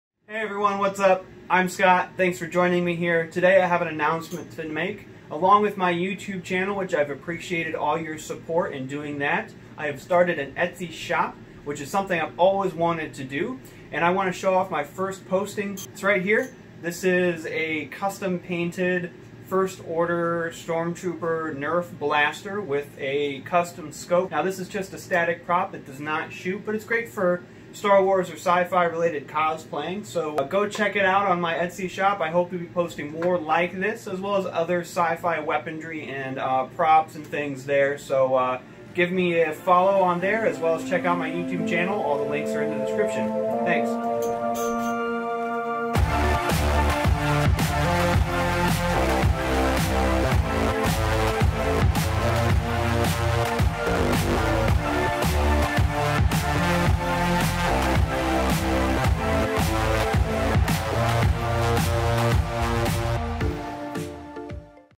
I've Started and Etsy Shop!
I'll be selling some of my custom sci-fi props and weapons there.  They're great for cosplaying!